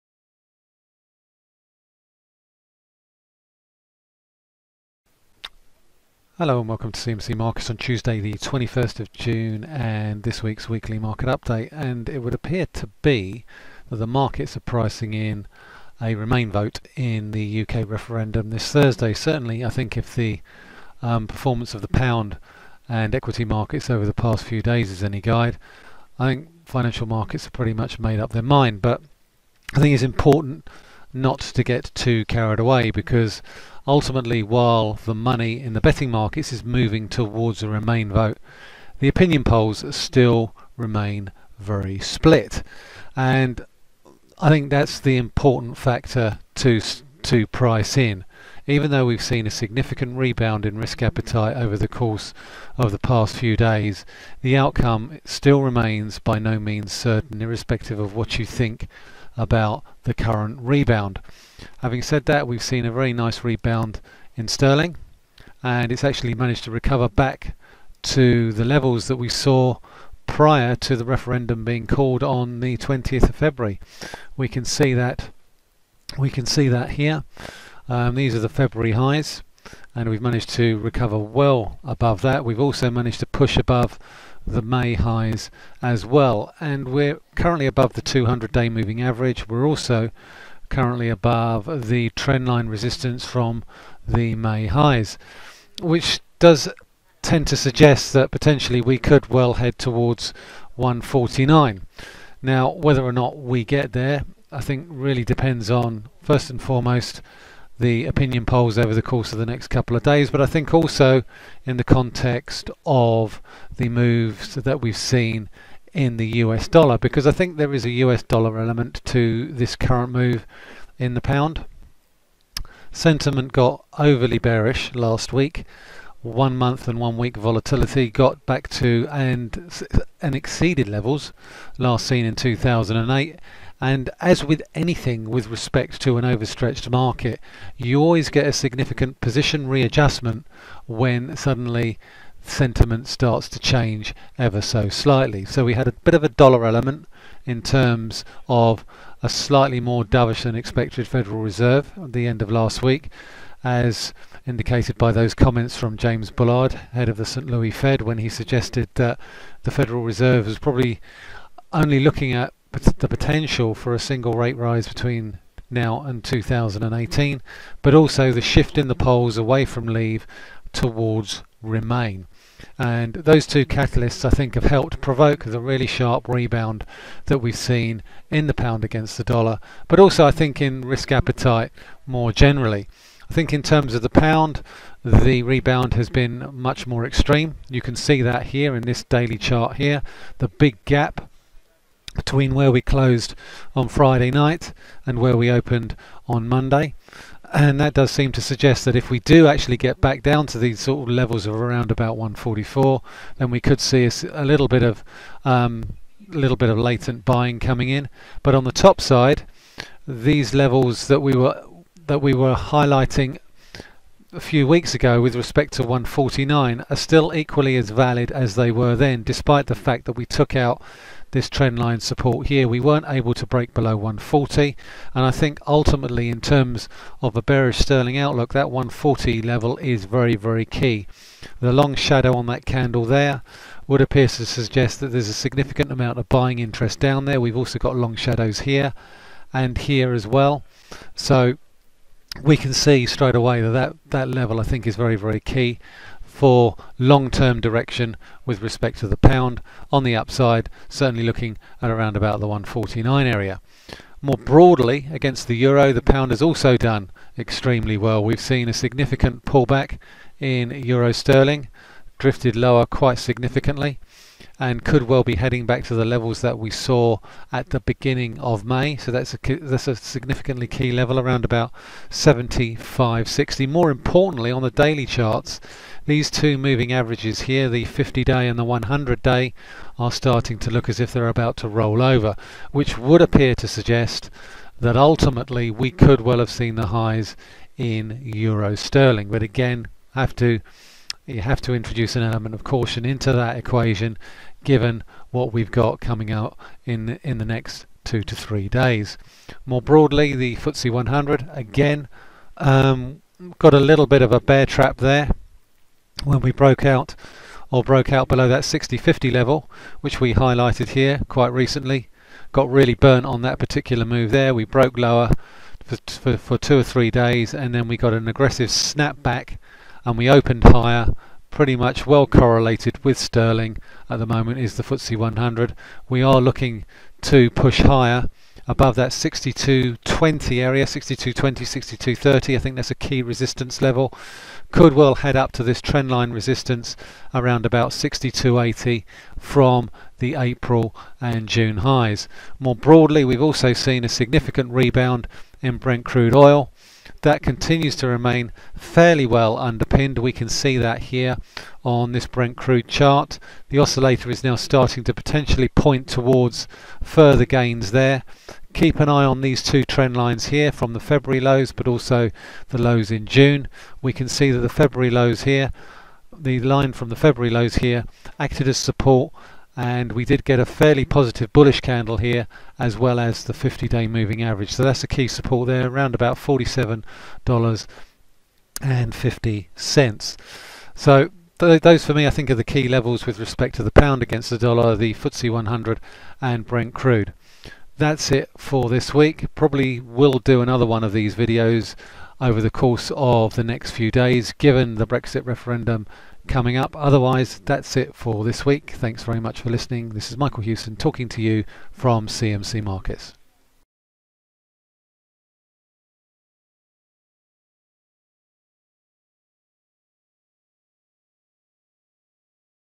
Is "Remain" a done deal as sterling and FTSE rebound - 21st June 2016
Michael looks at the sharp rallies in sterling and the FTSE100 and whether they are sustainable.